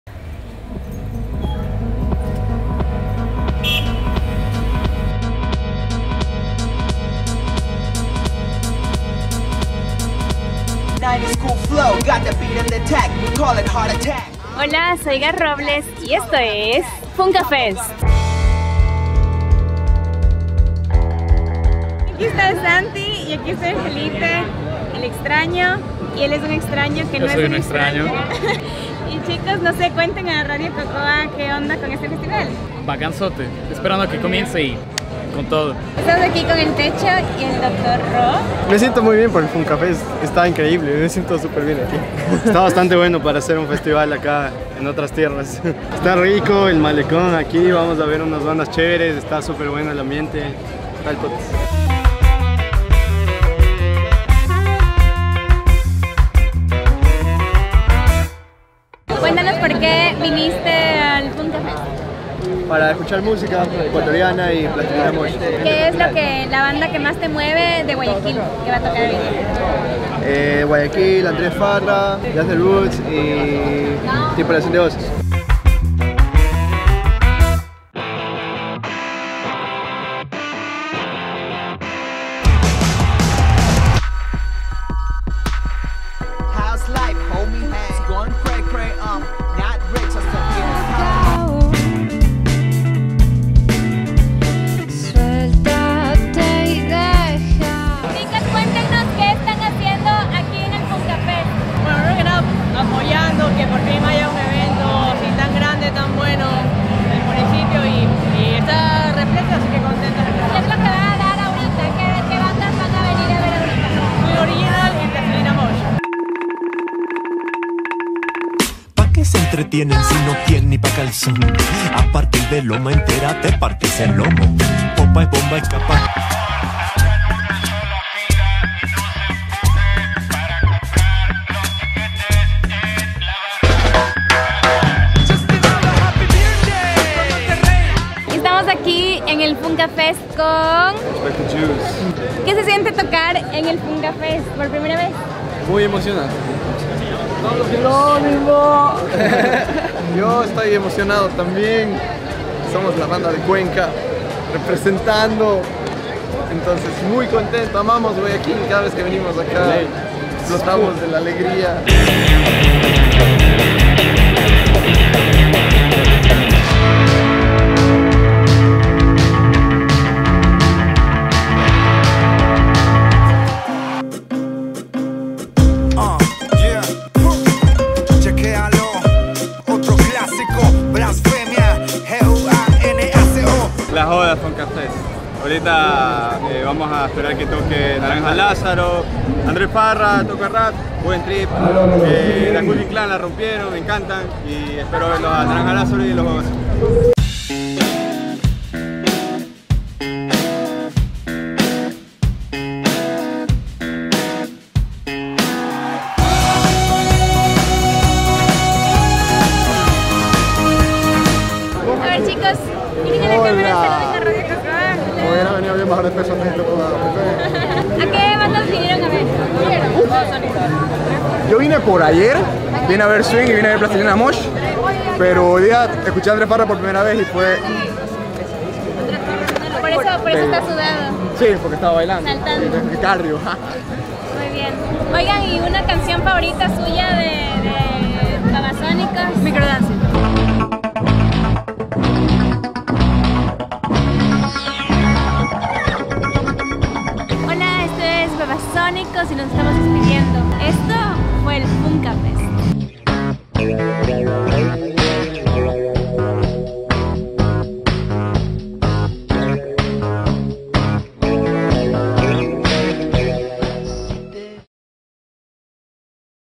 Funka Fest 2016 - Radio COCOA
Un recorrido por la primera edición del Funka Fest, un festival de 3 días en Guayaquil.
Fotografía: JJ Alomía.
Edición: Luis Paez.